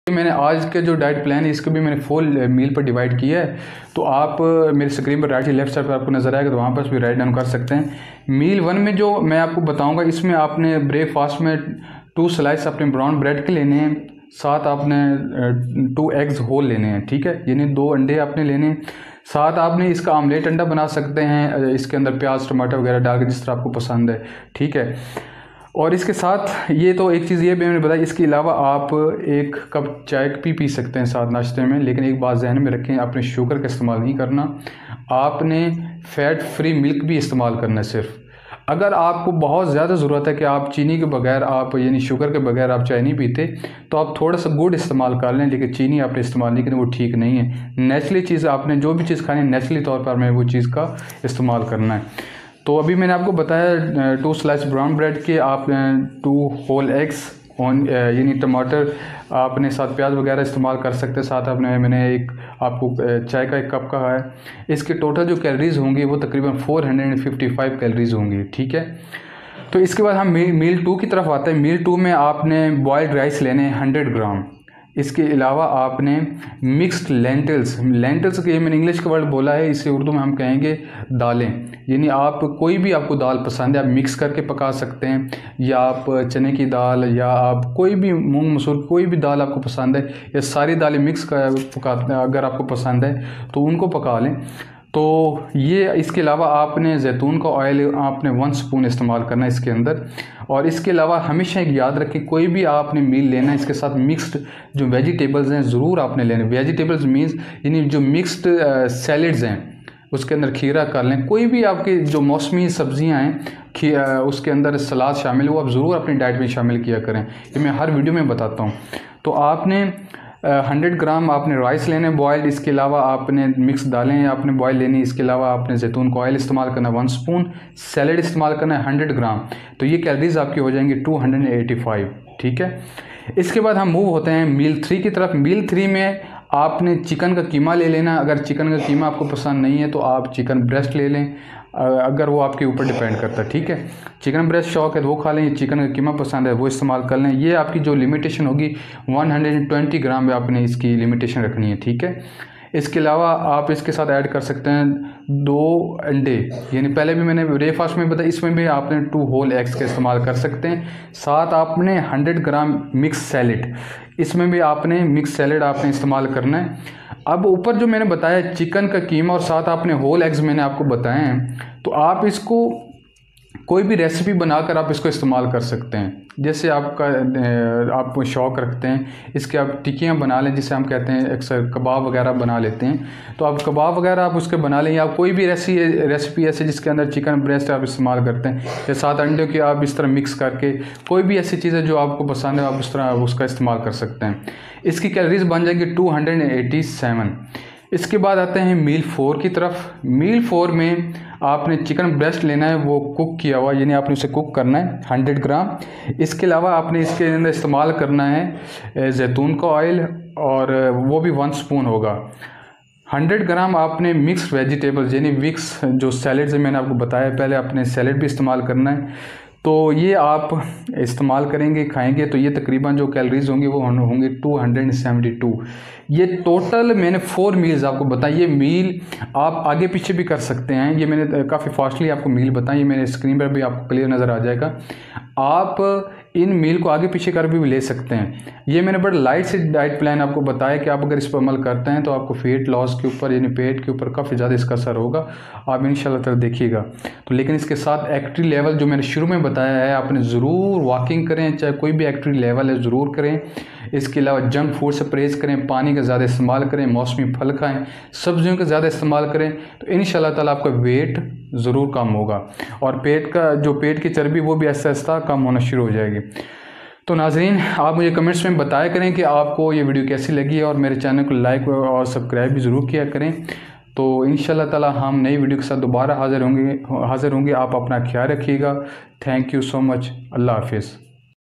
1200 Calorie Diet Plan For Quick Weight Loss
Full day meal plan for weight loss is best for women and men also.You will lose weight through this weight loss diet plan.

Meal 1

Bran Bread - 2 Slice
Whole Eggs- 2 Eggs
1 Cup Tea Without Sugar
Calories - 455

Meal 2

Boiled Rice - 100g
Boiled Lentils- 100g
Olive Oil - 1 Spoon
Mixed Salad - 100g
Calories - 285

Meal 3

Chicken Mince - 120g
Whole Eggs - 2
Mixed Salad - 100g
Calories - 285

Meal 4

Chicken Breast Cooked - 100g
Mixed Salad - 100g
Calories - 272